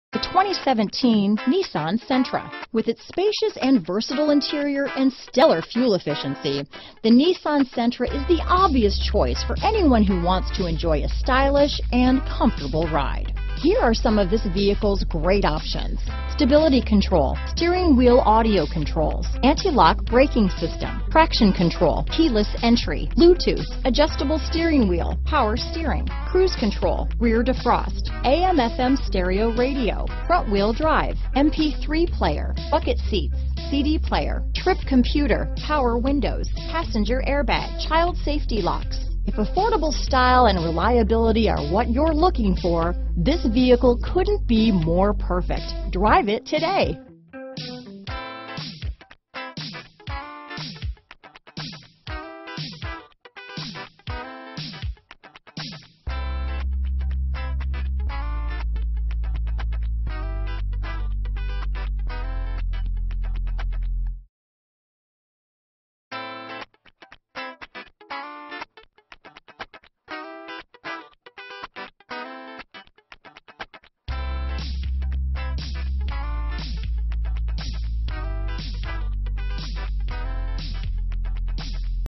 2017 Nissan Sentra Chicago, Matteson, Oak Lawn, Orland Park, Countryside IL 71357
[LISTING TYPE] 2017 Nissan Sentra

IIHS Top Safety Pick. Scores 37 Highway MPG and 29 City MPG! This Nissan Sentra delivers a Regular Unleaded I-4 1.8 L/110 engine powering this Variable transmission. Window Grid Antenna, Wheels: 16 Steel w/Full Wheel Covers -inc: 10-spoke design, Variable Intermittent Wipers.*This Nissan Sentra Comes Equipped with These Options *Urethane Gear Shift Knob, Trunk Rear Cargo Access, Trip Computer, Transmission: Xtronic CVT, Torsion beam rear suspension w/coil springs, Tires: P205/55R16 AS, Strut Front Suspension w/Coil Springs, Steel Spare Wheel, Single Stainless Steel Exhaust, Side Impact Beams.* Visit Us Today *Stop by Western Avenue Nissan located at 7410 S Western Ave, Chicago, IL 60636 for a quick visit and a great vehicle!